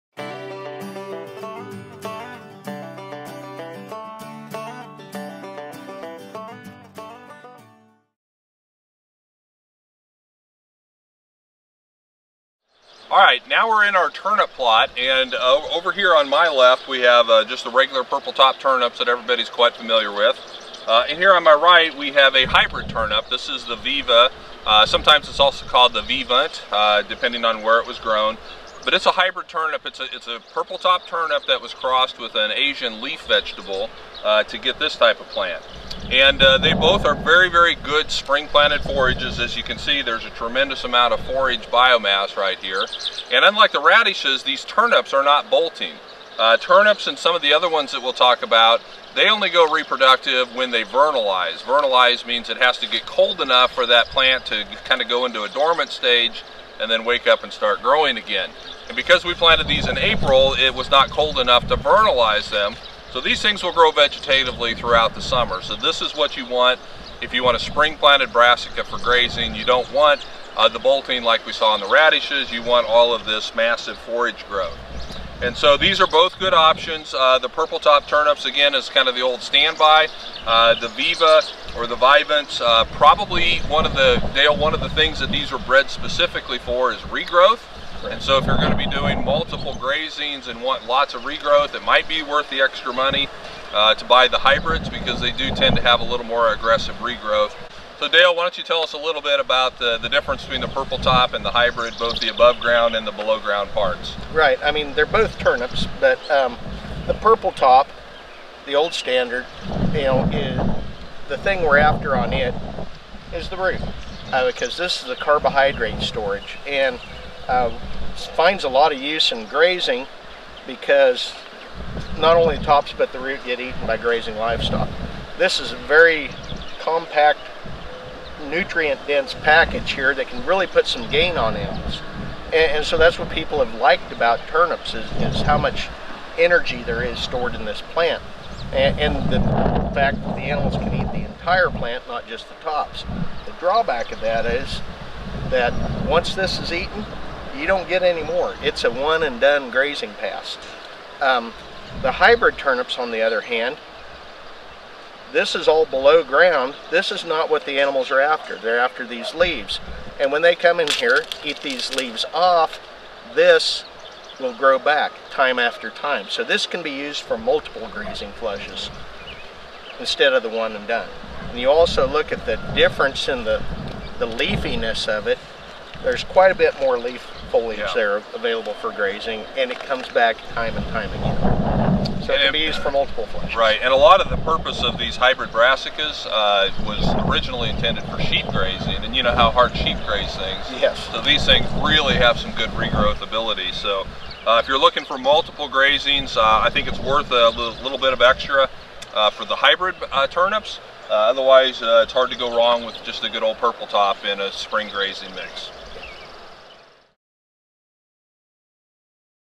Purple Top Turnip and Vivant Hybrid Turnip - Test Plots 2019
Keith and Dale talk about Turnips!